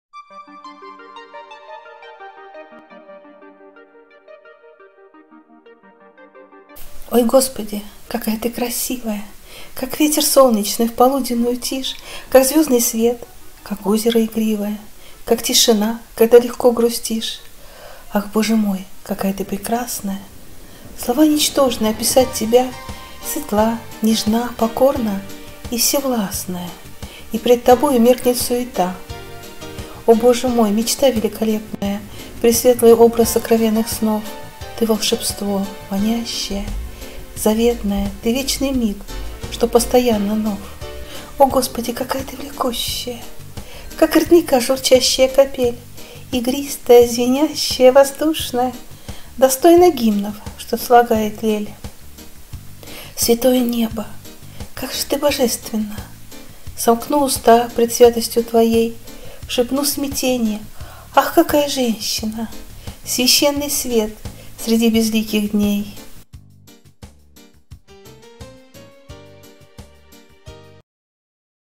Восторженность. Ой, Господи! Какая ты красивая! Автор стиха Игорь Арефьев. Стихи о любви.
Современная поэзия.